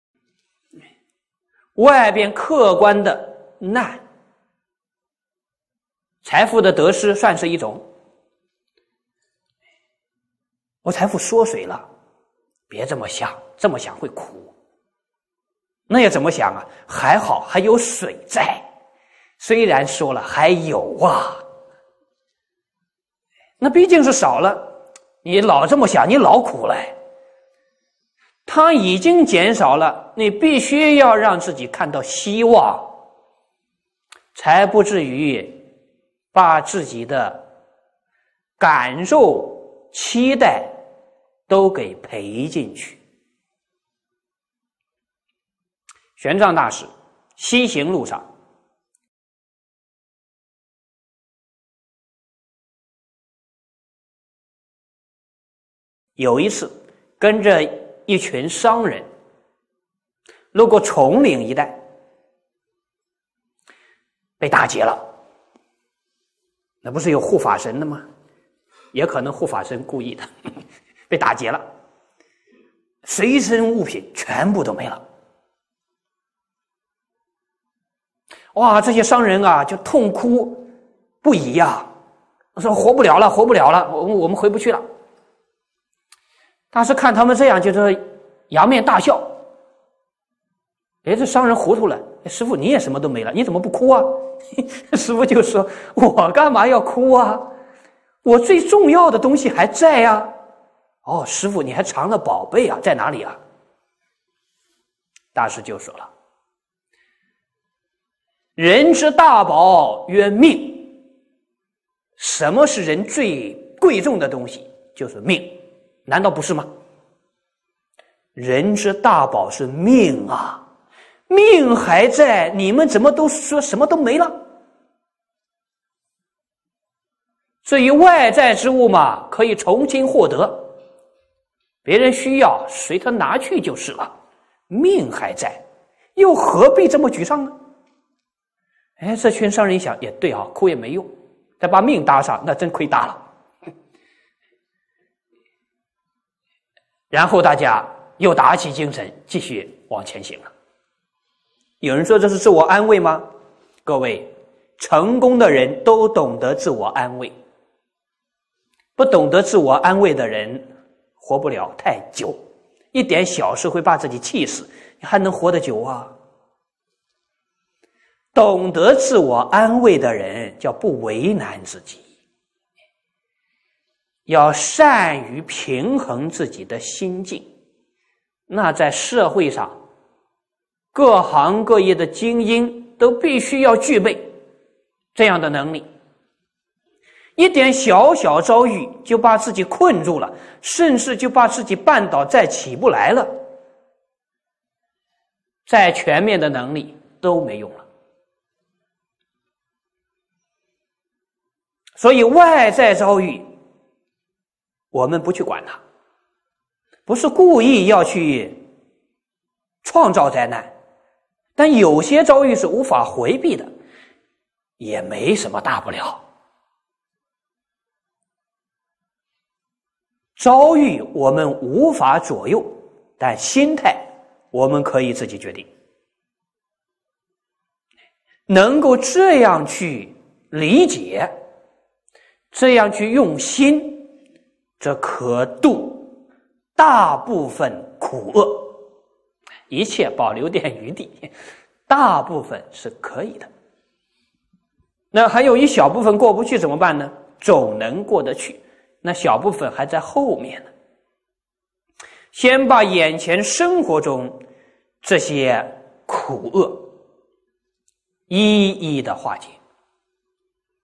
Master Ren Shan - Every Successful Person Has this Ability 每一個成功人士都具備這樣的能力
(English subtitles) Master Ren Shan 仁山法師, born in 1980, is a remarkable Buddhist master in China.
Subtitles:
One of the external objective disasters is the gain or loss of wealth. My wealth shrinks like clothes shrinking in water. Don't think like this, it will make you miserable. Then how should I think of it? Luckily, there is something left. Though it's narrowed down, you still have it. After all, it got less. If you always think like this, you'll be miserable forever. It has diminished and you must let yourself see hope so that it won't let you down and make you depressed. In the past, Master Xuan Zhuang went westward. On one occasion, he and a group of traders were robbed as they passed through the woods.
1:00
Should there be a Dharma patron? Maybe he was deliberate. All their belongings were gone. All these businessmen couldn't stop crying. We couldn't live without these things. We have nothing left. Seeing them so sad, the Master raised his head and laughed. These businessmen were confused and said: Master, you have nothing left too. Why don't you cry? Master said: Why should I? I still keep the most important thing. Master, you have hidden a treasure, where is it? Master said: The greatest treasure for a person is his life. What's the most precious thing for a person?  That's his life. Isn't it? You are still alive. Why do you say: We have nothing left?
2:00
You can regain those external things. If people are in need, just take it. Since there is still life, why should we be sad? These businessmen thought it was true, crying wouldn't help. If I add the loss of life, it will be a huge loss. Then everyone cheered up and continued their journey. Some people say this is self-comfort. All successful people know self-comfort. People who don't know self-comfort won't live long. A mere trifle can drive you crazy. How can you live long? Those who know self-comfort will not embarrass themselves. You need to balance your mindset. This ability is needed by the elite in every sector of society.
3:00
One small encounter can get you into trouble or even trap you, and you can't get up. Even if you have abilities in all aspects, it doesn't help. So, you should ignore external encounters. It’s not about being careless about risky situations. But some events are unavoidable. It's not a big deal. We can not change encounters, but we can decide our mindsets. If you can realize this and be mindful of this, you can be freed from most misery. I leave some room. Most of them. But there is still a small portion that cannot be passed. what to do? You will pass that later. First, solve the current pains individually in daily life.